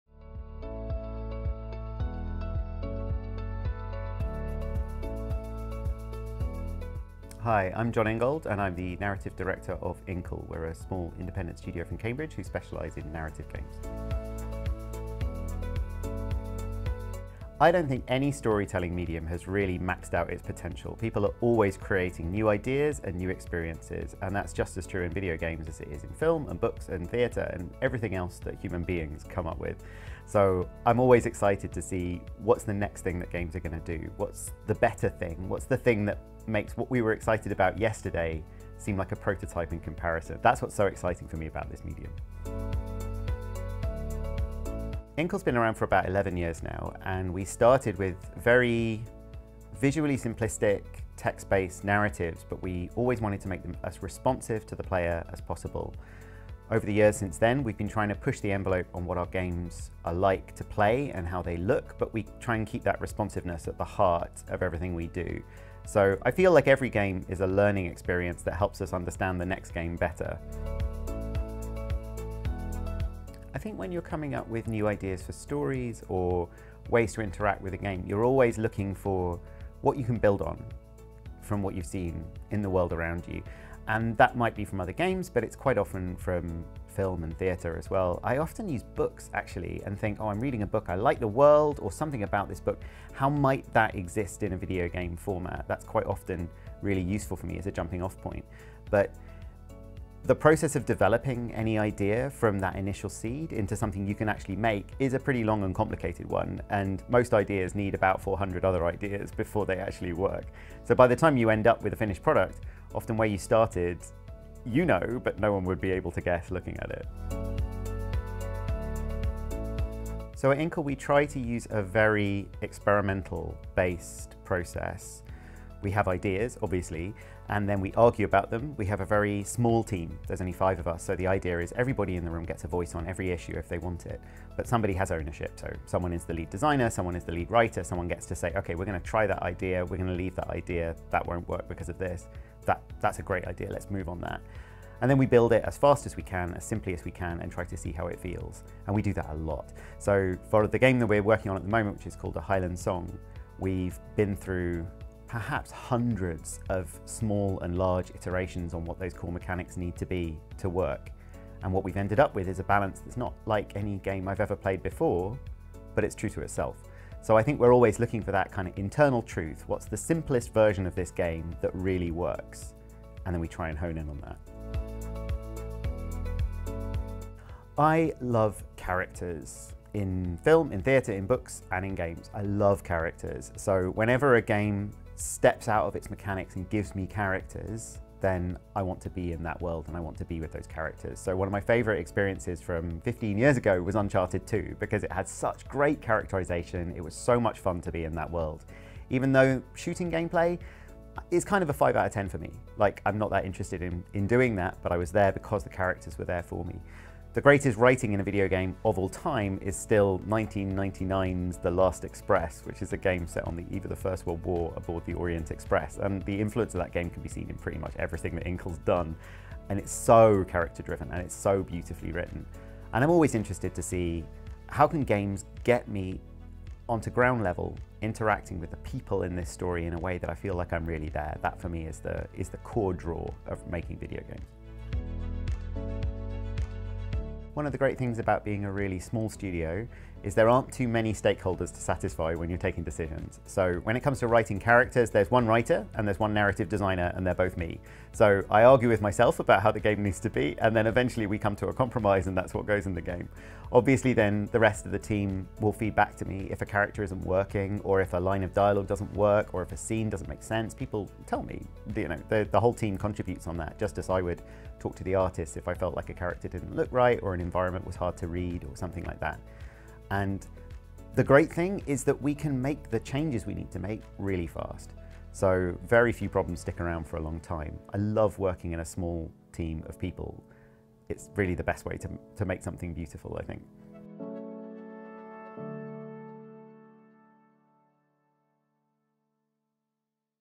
Inkle's Jon Ingold | Develop:Brighton 2023 Keynote | Backstage Interview
Jon Ingold is interviewed after his 2023 Keynote at Develop:Brighton